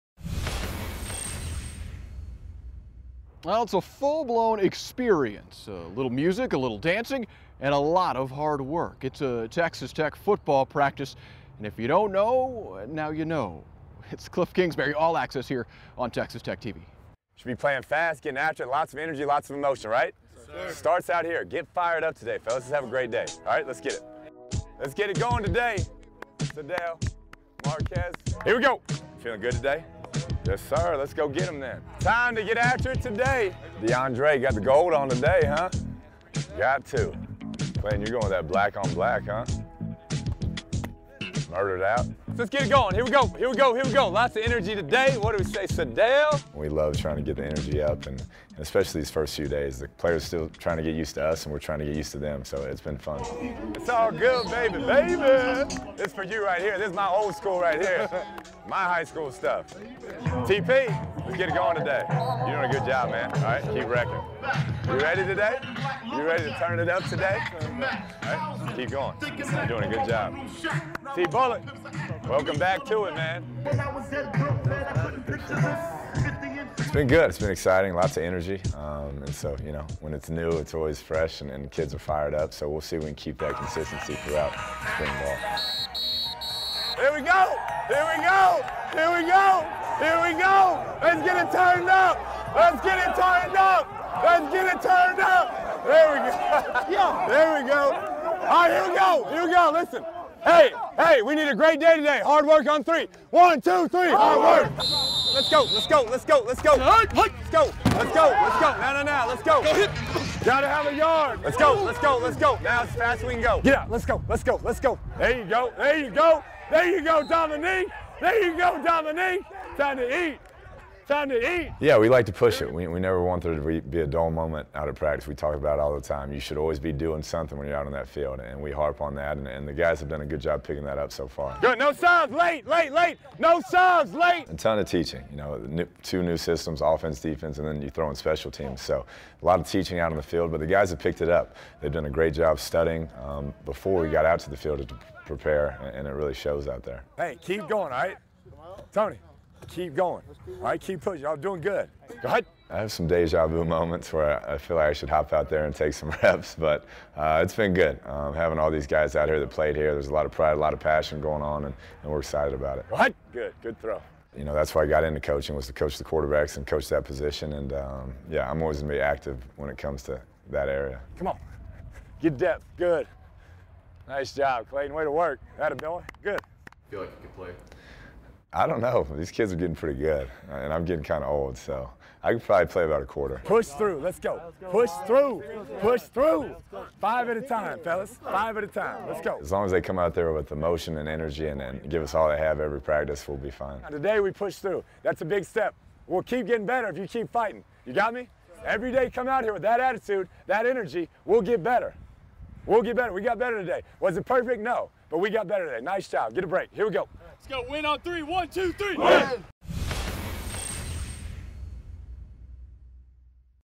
Texas Tech Football: All-Access: Kliff Kingsbury | 2013
Take a behind-the-scenes look at Texas Tech Football Practice with Kliff Kingsbury.